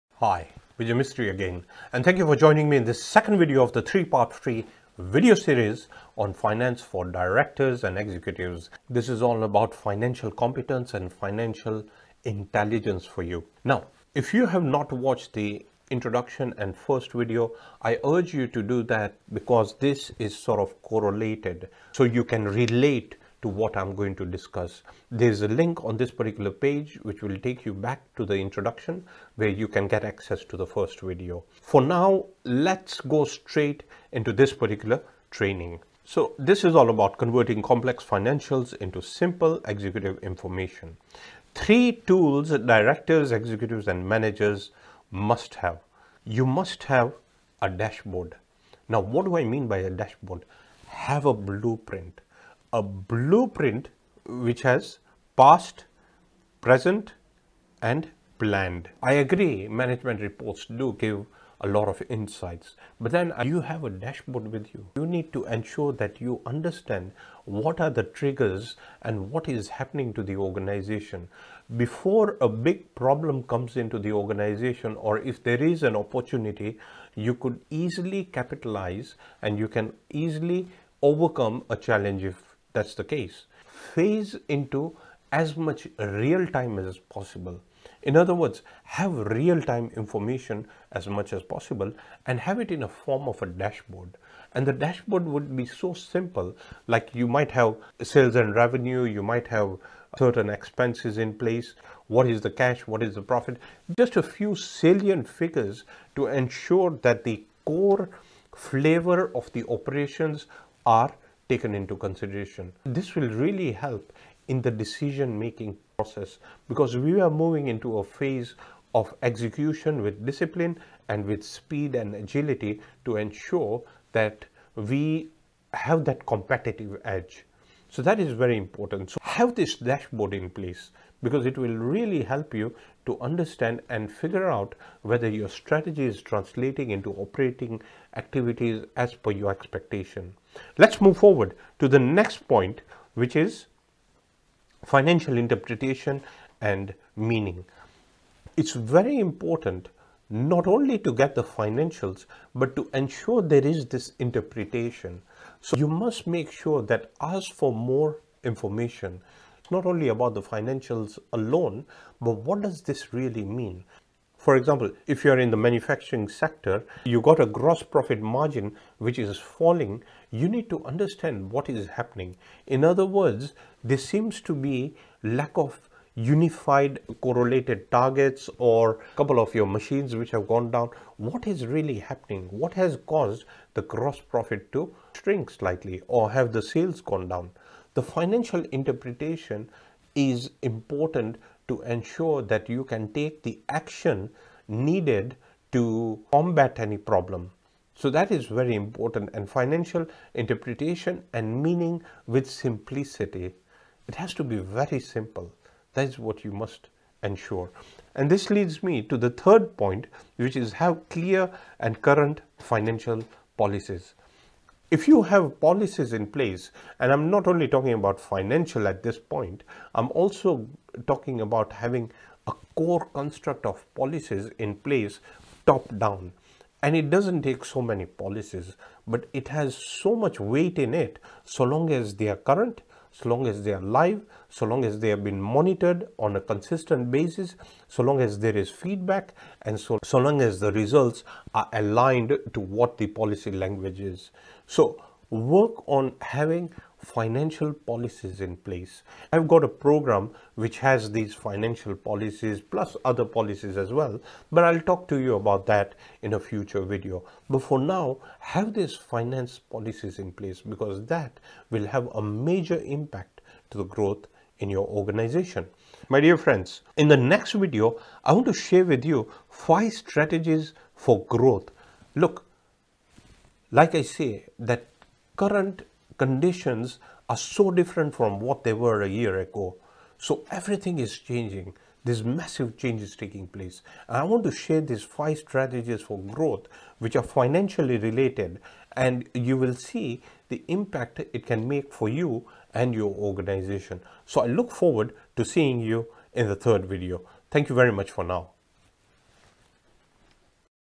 Finance Training for Directors, Executives and Managers - 2nd Video
This Finance Training for Directors, Executives and Managers is the second in a three part video series. In this video, I share three tools which Directors, Executives and Managers must have. Firstly, they must have a financial dashboard revealing key information which will have a direct impact to certain core areas of your business. This Finance training for directors, executives and managers further reveals the importance of financial interpretation and clear meaning behind why certain results occurred. And finally it discusses the importance of having a few policies which would further drive growth.
if you would like to have the full finance training for directors , executives and manager's program which we believe to be cutting edge.